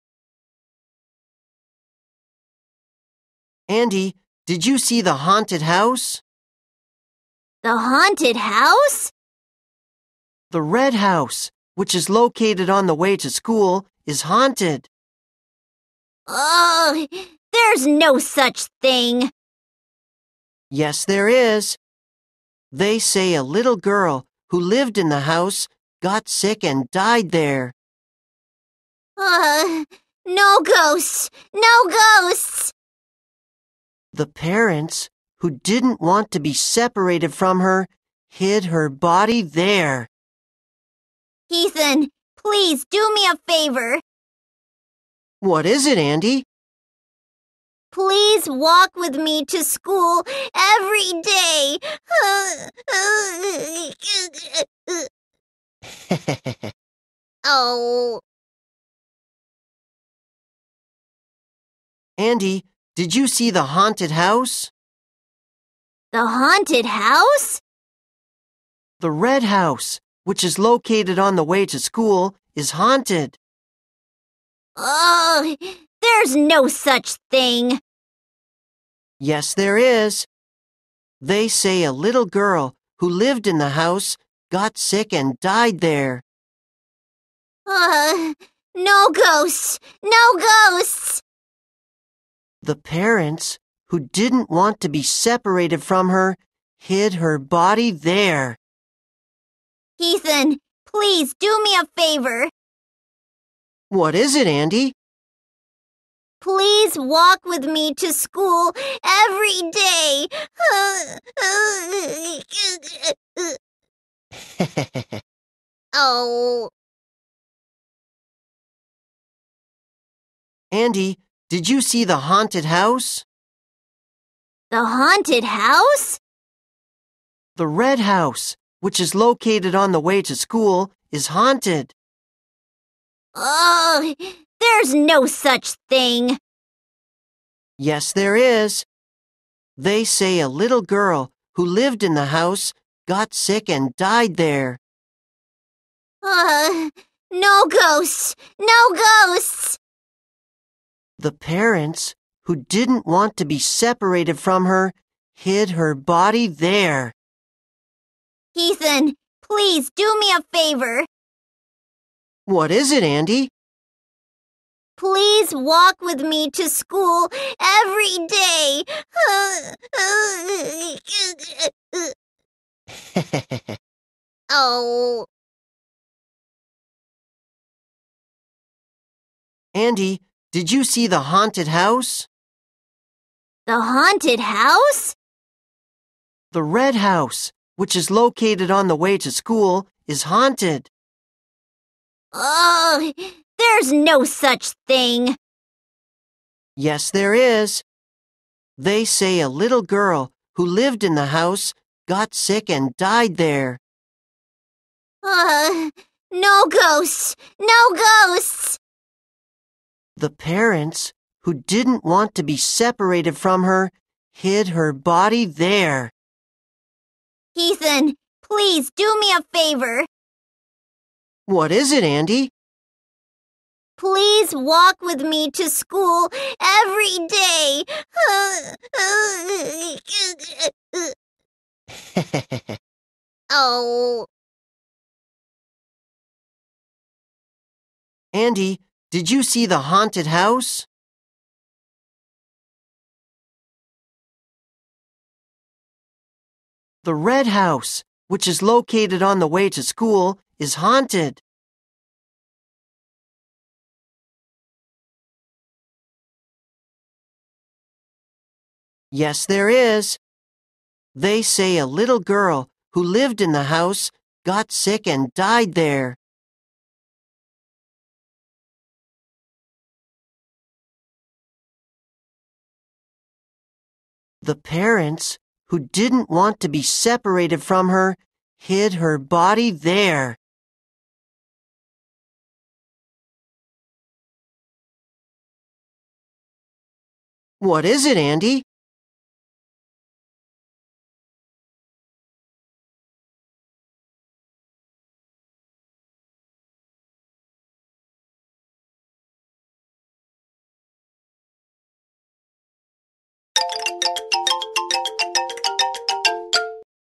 [반복반복] 영어회화 87 - 관계대명사 who 와 which 의 표현법 알기 심화 3편
2번 보고 듣고, 영어 스크랩트 없이 1번 또 보고 듣고, 모든 스크립트 없이 1번 더 보고 듣고, 마지막은 주인공 역할로 말하기(롤 플레잉)
Andy, did you see the haunted house? 앤디, 유령이 나오는 집을 봤니?
The haunted house? 유령이 나오는 집?
The red house, which is located on the way to school, is haunted. 학교 가는 길에 있는 빨간 집에 유령이 나온대.
(Argh!) There's no such thing! 그런게 있을리 없어!
Yes, there is. 아냐, 있어.
They say a little girl, who lived in the house, got sick and died there.  그 집에 살던 어린 여자 아이가 아파서 죽었대.
(Argh!) No ghosts! No ghosts! 유령은 없어! 유령은 없다고!
The parents, who didn't want to be separated from her, hid her body there. 그 아이와 떨어지기 싫었던 부모가 그곳에 그 아이의 시신을 숨겼다나봐.
Ethan, please do me a favor! 에단, 내 부탁 좀 들어줘!
What is it, Andy? 뭔데, 앤디?
Please walk with me to school everyday! (sobs) 제발 나랑 매일 학교에 같이 가줘!